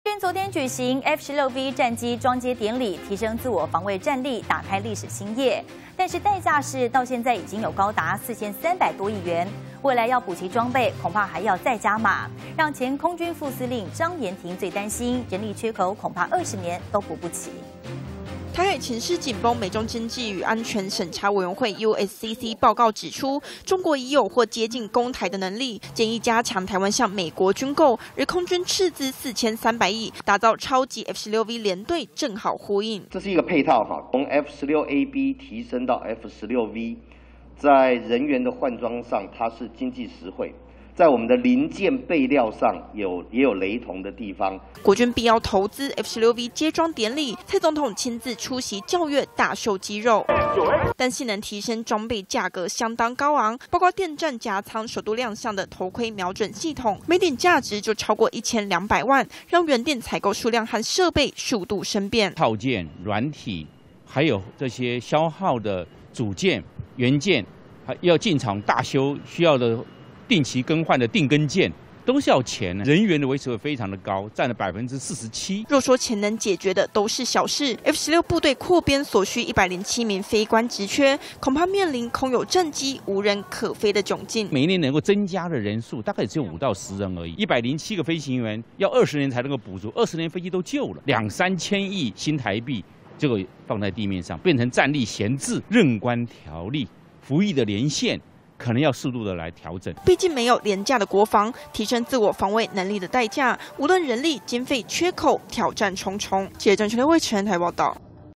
斥資4300億造F-16V戰隊!前軍官憂”無人飛”
美中經濟與安全審查委員會USCC報告指出，中國已有或接近攻台的能力，因應台海情勢，空軍昨天(18)舉行F-16V戰機接裝典禮，提升自我防衛戰力，蔡英文總統到場校閱，揭開空軍歷史新頁，但打造超級F-16機隊的代價，至今已高達4300億多元，未來要補齊裝備，恐還要加碼，前空軍副司令張延廷最擔心，人力缺口恐怕20年都補不齊，可能面臨無人可飛的狀況。